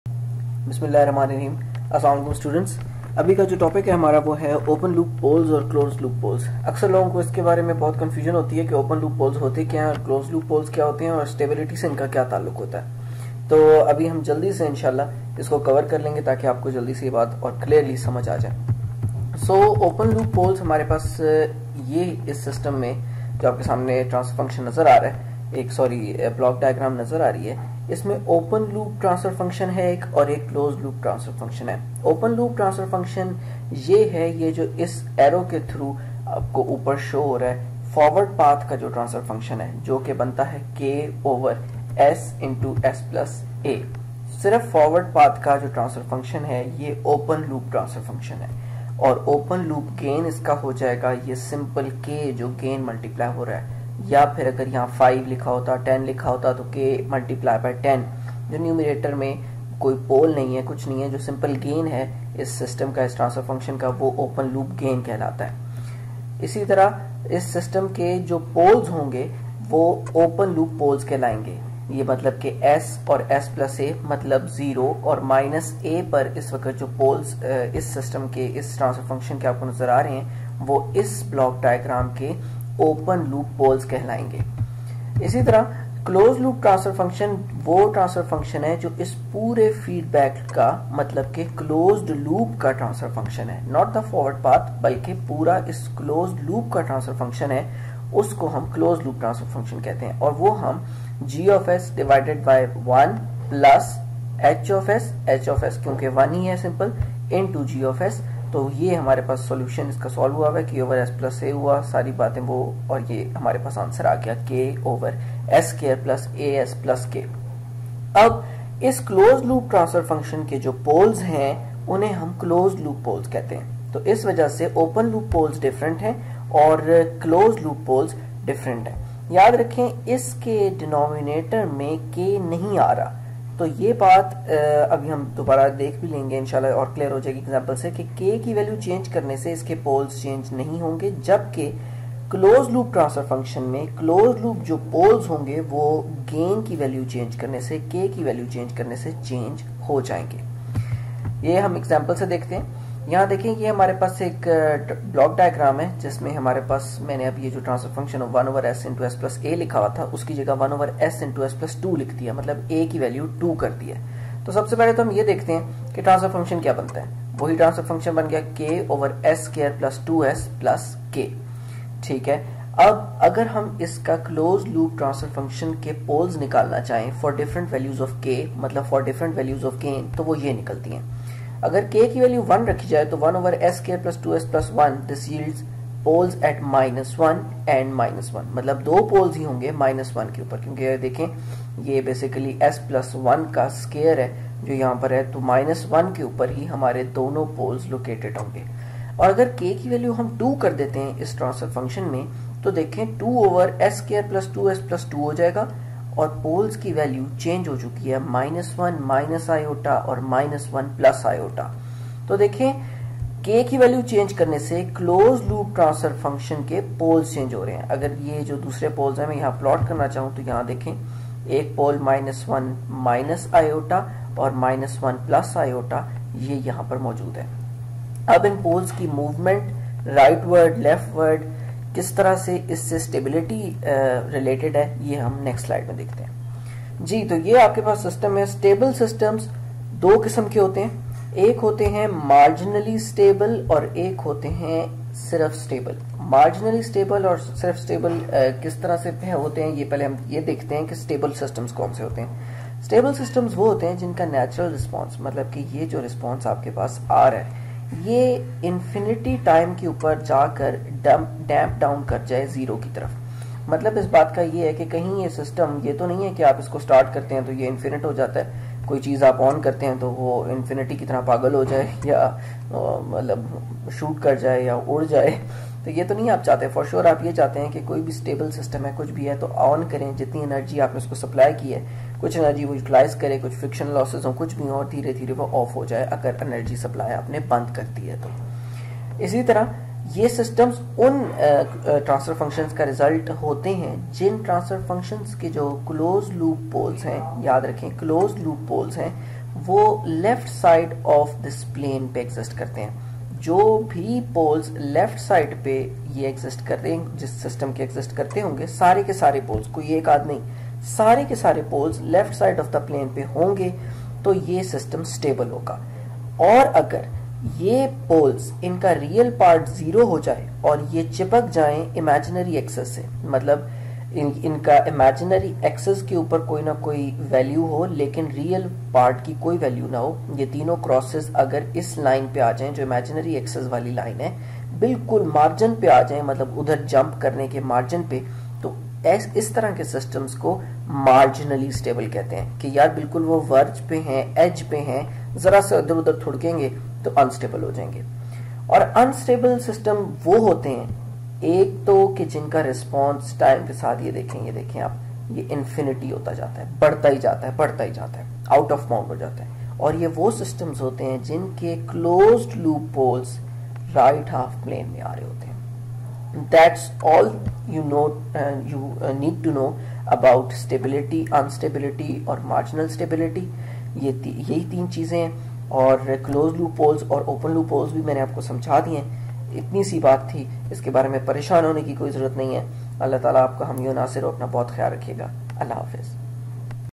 Open Loop poles Closed Loop poles and Stability | Control Systems | Urdu/Hindi Lecture
Open Loop poles Closed Loop poles and their relation with Stability has been explained in this short video in detail. Do mention your questions/queries in the comments section.